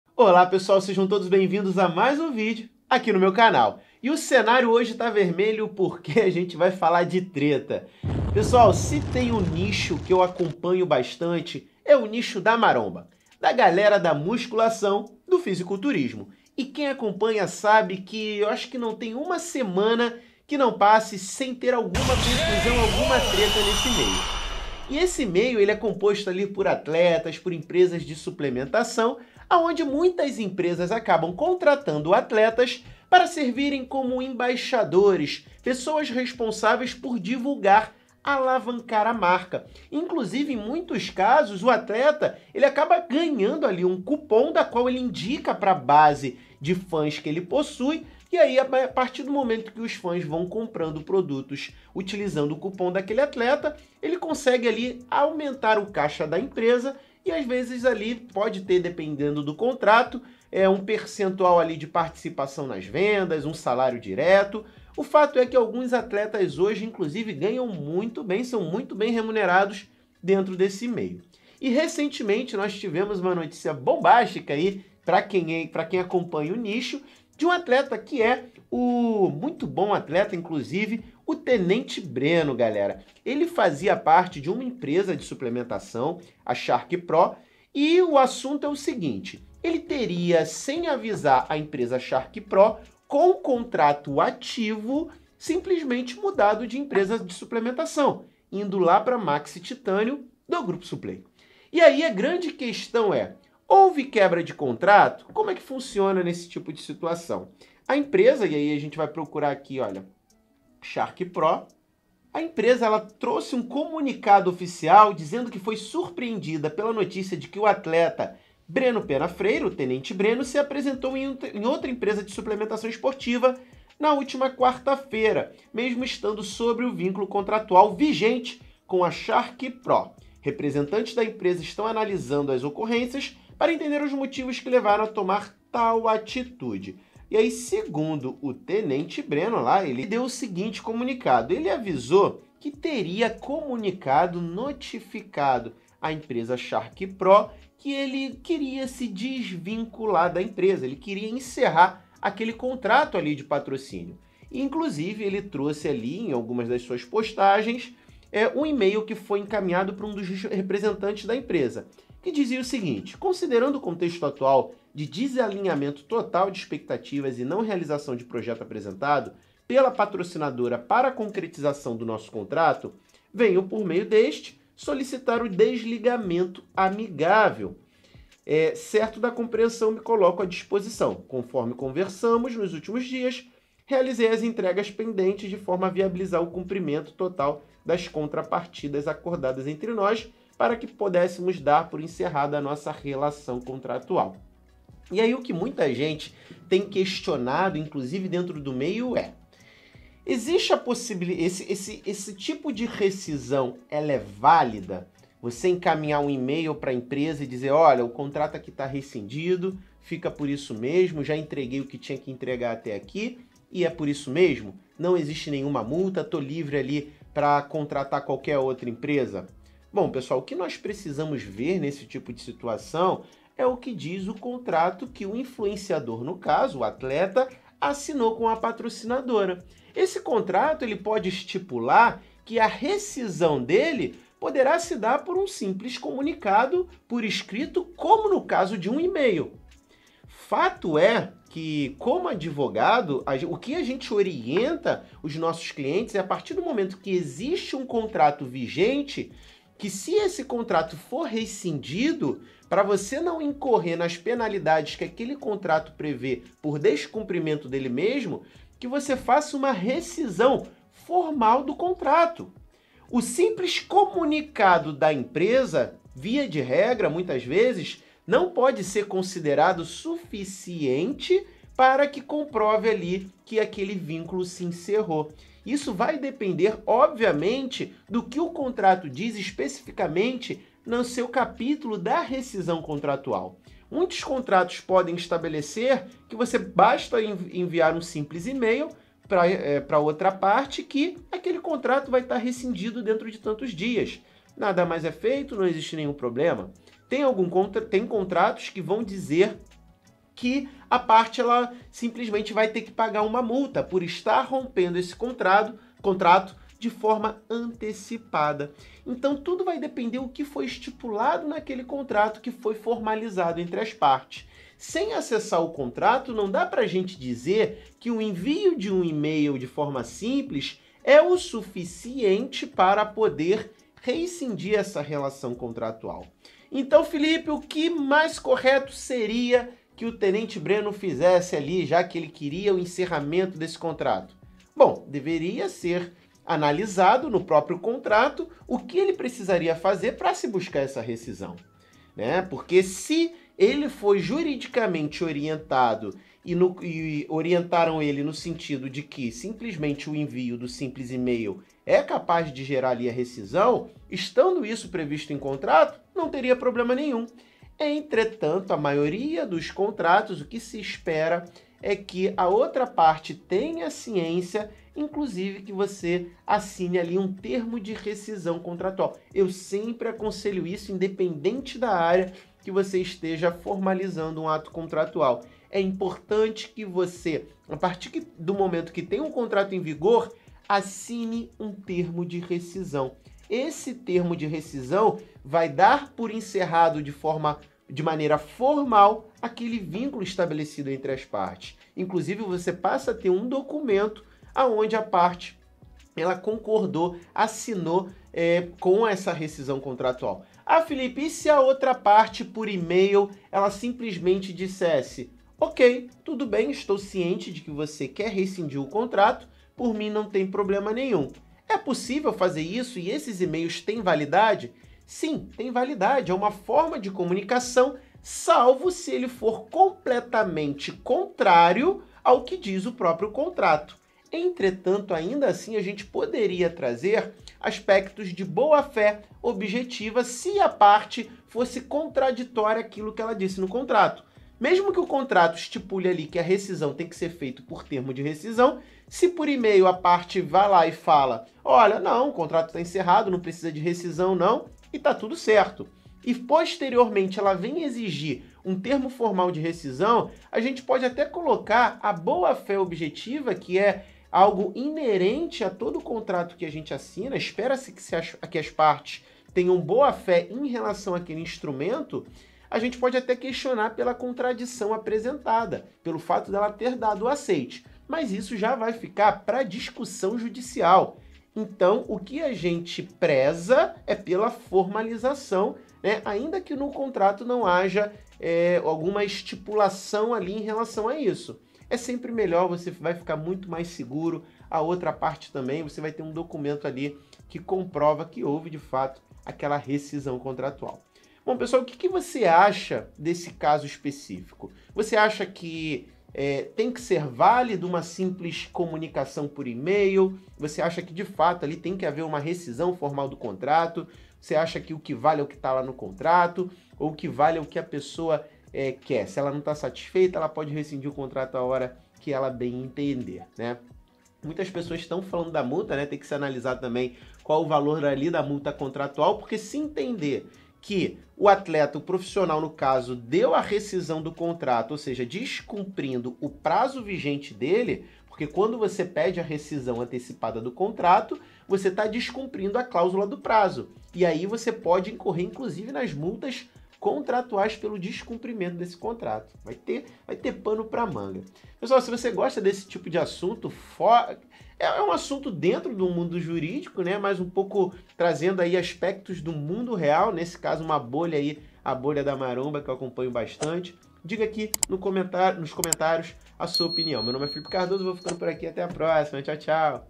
Fisiculturista Choca Patrocinadores: Rompe Contrato com Um Email
Neste vídeo explosivo, mergulhamos fundo na controversa decisão do Tenente Breno, um renomado fisiculturista, que sacudiu o mundo do fisiculturismo ao romper seu contrato com a Shark Pro Suplementos por meio de um simples email.

📌 Sua Opinião Importa:
Após assistir, deixe seu comentário abaixo. O que você acha da decisão do Tenente Breno? Qual deve ser o próximo passo para ele e para a Shark Pro? Sua opinião é valiosa para nós!